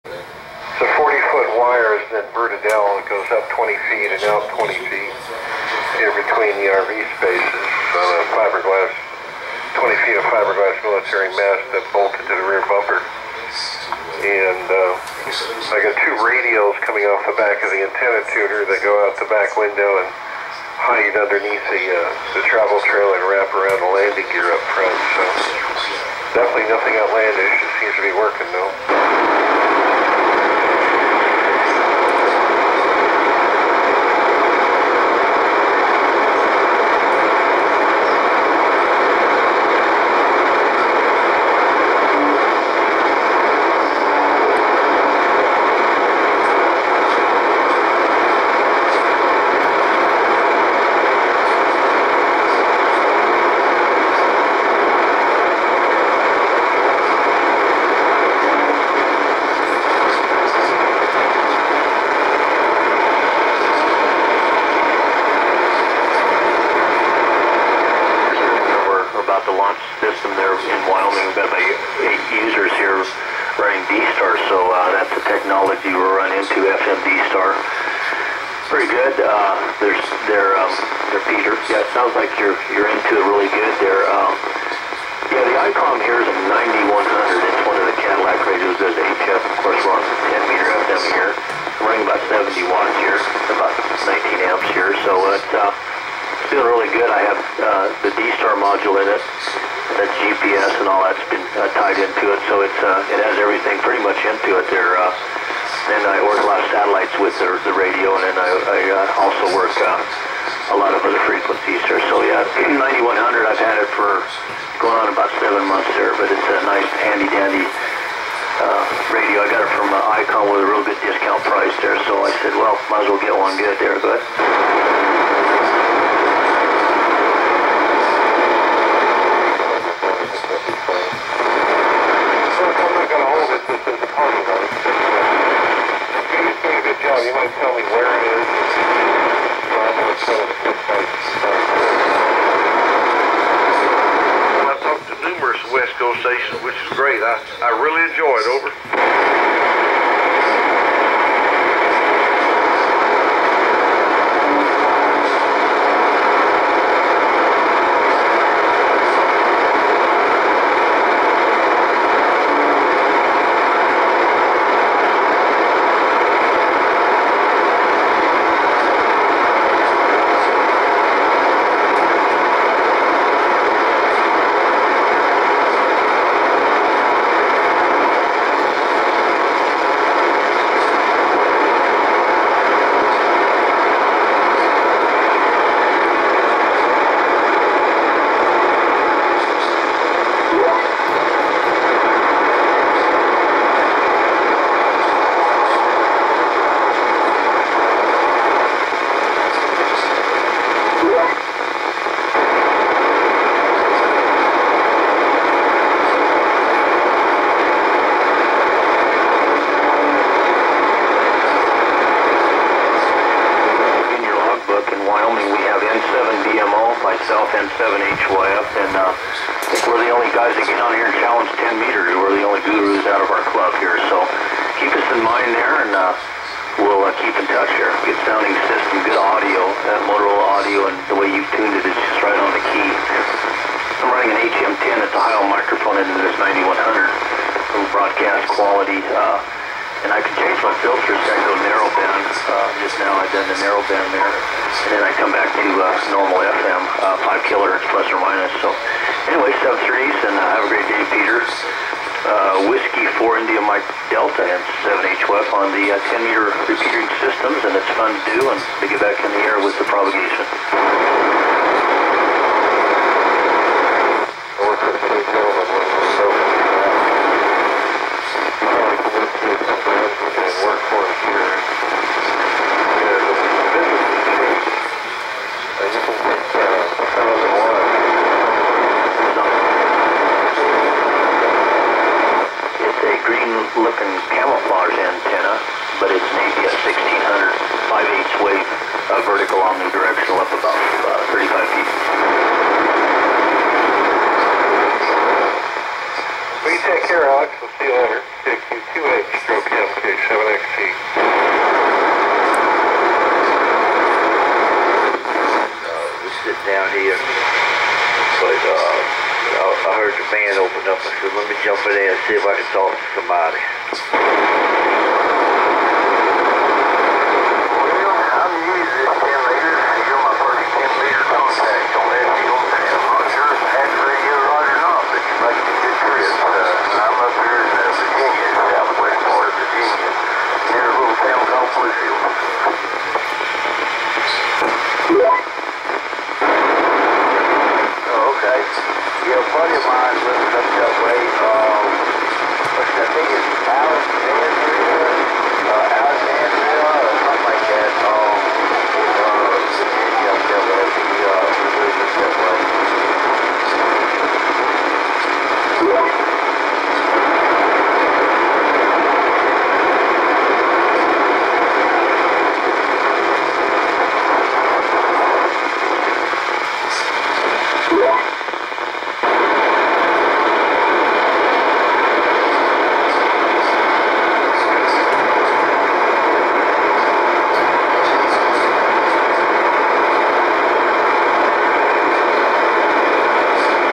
10 meters amateur FM opening october 17th 2013
Great opening of the 10 meters amateur radio band